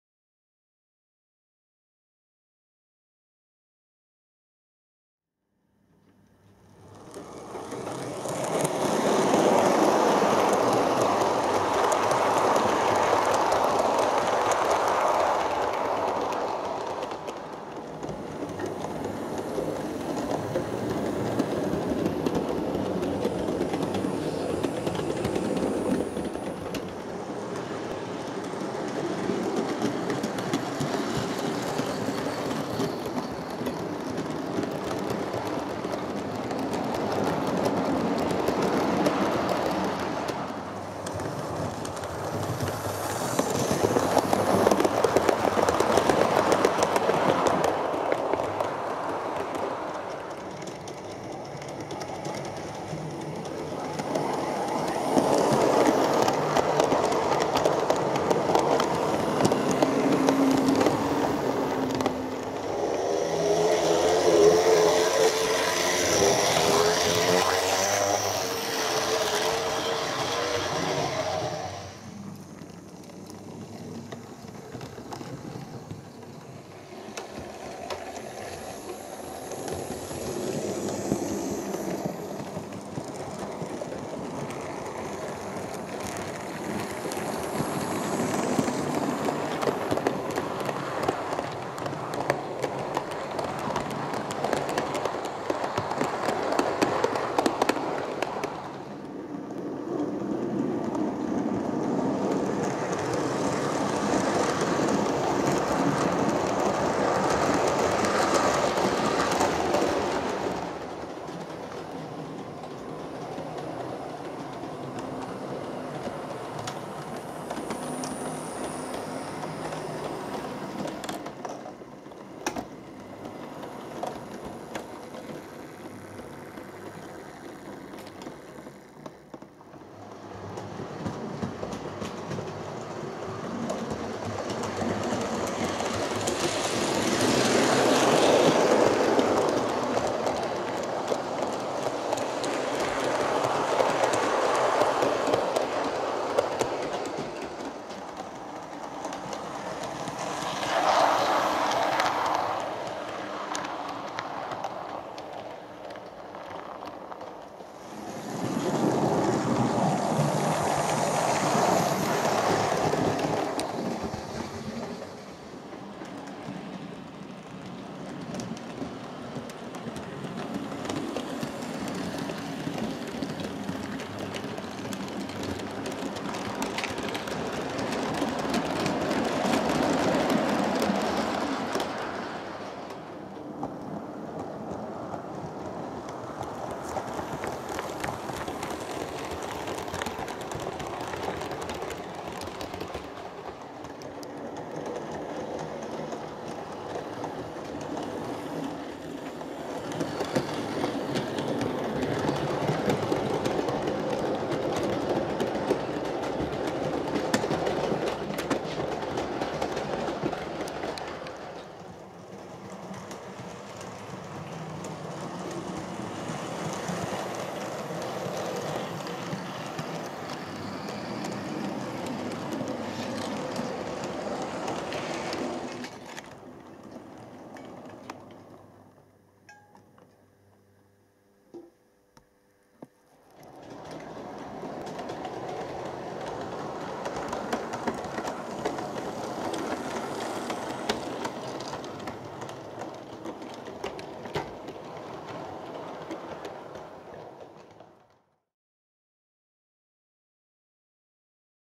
RSR Santa Specials & Christmas Day Specials
A short video of our Santa Specials and Christmas Day Specials this year. A Variety of locos were out in action:

- GWR 5101 Class - 5164 (From the SVR)
- GWR 3700 Class - 3717 'City of Truro'
- BR Standard 4 Mogul - 76109 (From owner Michael Wells)
- BR Standard 4 Class - 75033
- BR (W) 1600 Class - 1638
- LT Pannier Tank - L.94
- BR Class 47 - D1572
- BR Class 35 - D7062